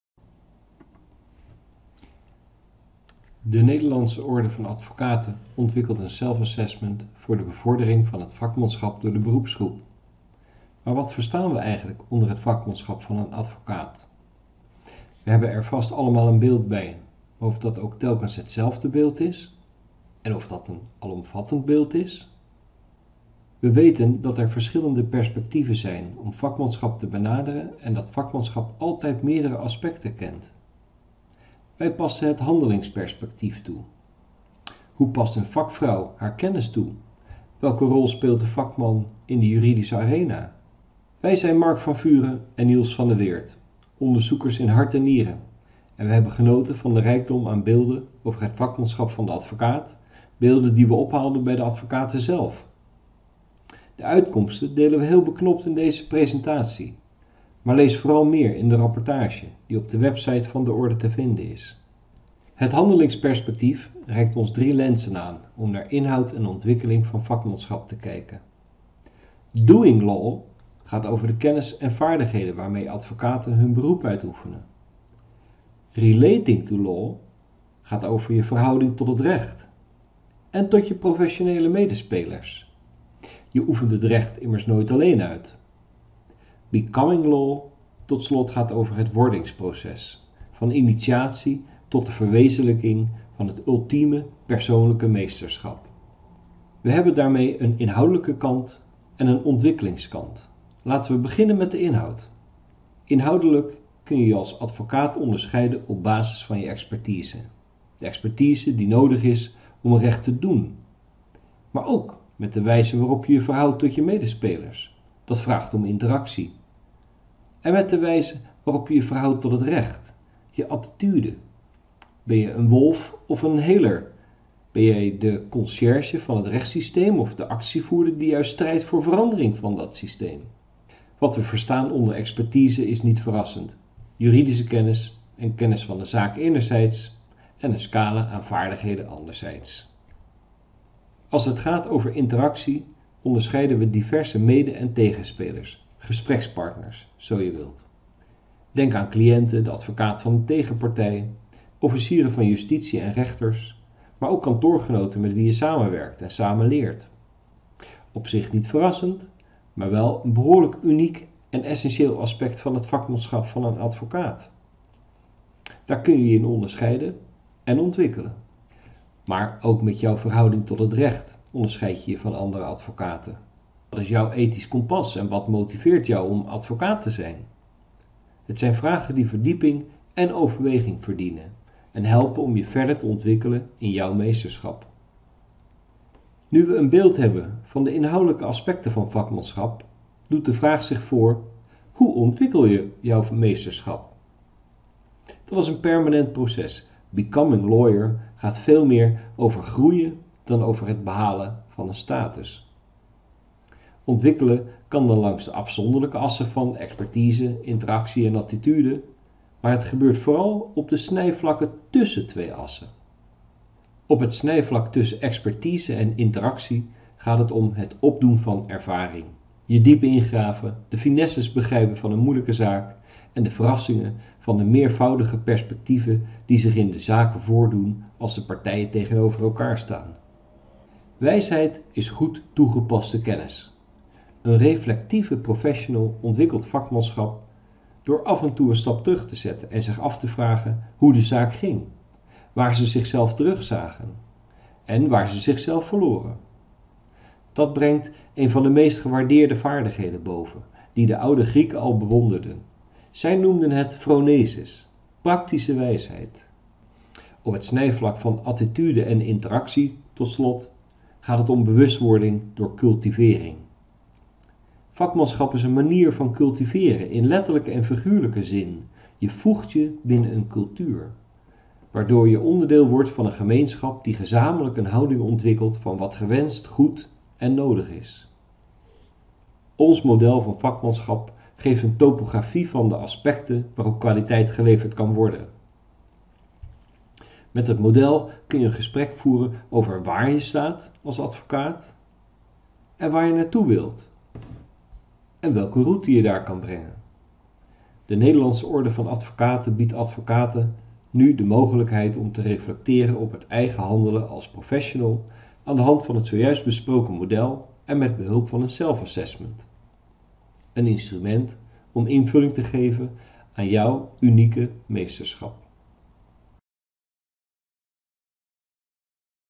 Meesterschap is vakmanschap
Om de kwaliteit van de advocatuur te bevorderen heeft de Nederlandse orde van advocaten een digitale self-assessmenttool ontwikkeld. Hiermee krijgen advocaten geheel vrijwillig en kosteloos de mogelijkheid om te reflecteren op hun eigen handelen als professional. Wetenschappers van de Erasmus Universiteit en de Universiteit Twente hebben in dit kader een verkenning uitgevoerd naar het vakmanschap van de advocaat. De onderzoeksresultaten zijn gebundeld in het rapport Meesterschap is vakmanschap. In deze presentatie geven beide onderzoekers inzicht in dit model dat aan de self-assessmenttool ten grondslag ligt.